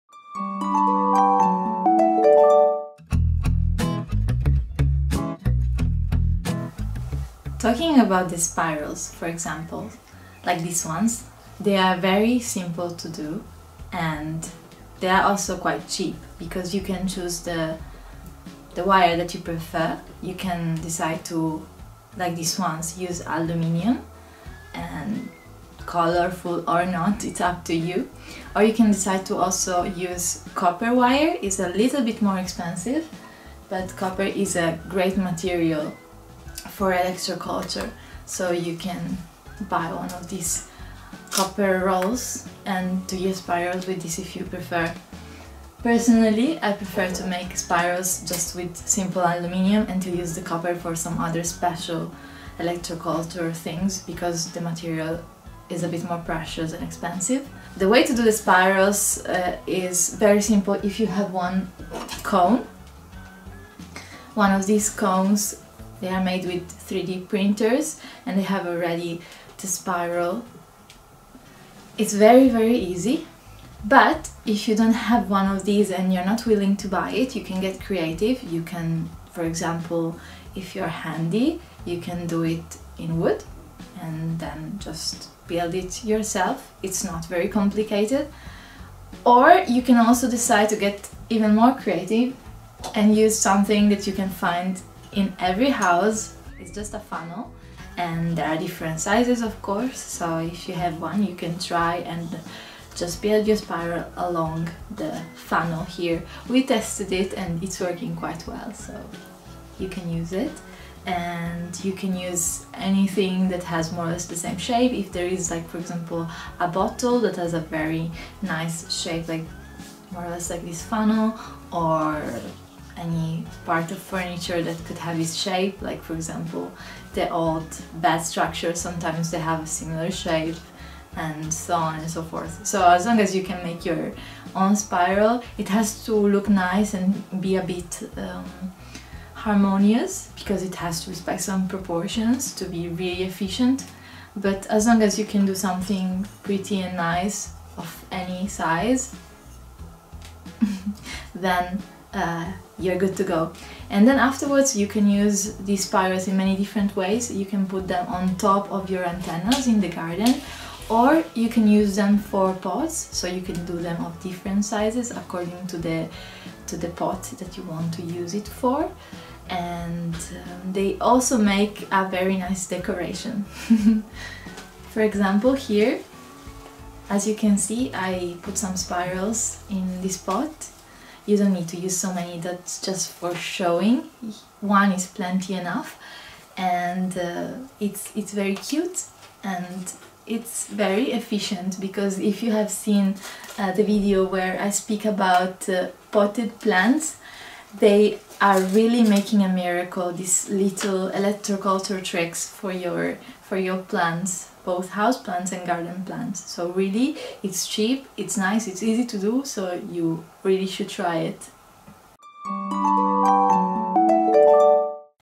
All about SPIRAL CONES in Electroculture | Ighina spirals of Lakhovsky
The spiral cones are one of the most symbolic elements in modern elctroculture. Here we give we explain to you all you should know about them. It is easy an a cheap to make them by yourself!